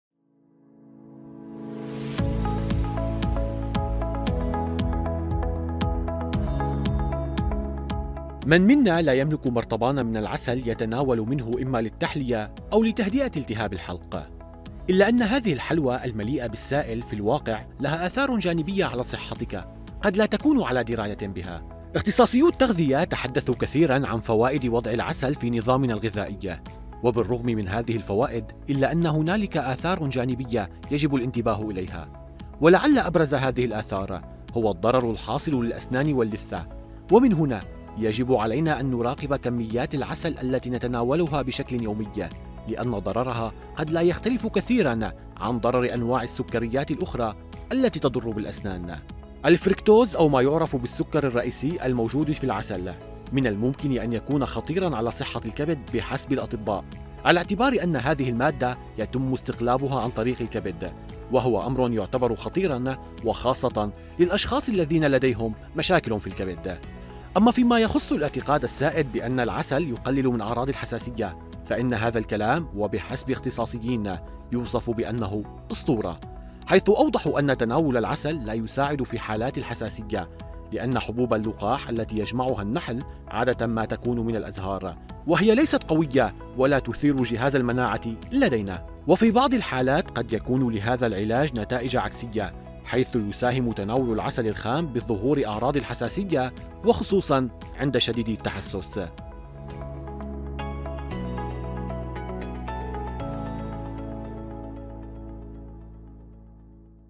اعراض جانبية لانعرفها عن العسل| برنامج دامسكو
الأعراض الجانبية للعسل وكيف ممكن  أن يأثر علينا رغم أنه مادة مفيدة
 الموقع الالكتروني: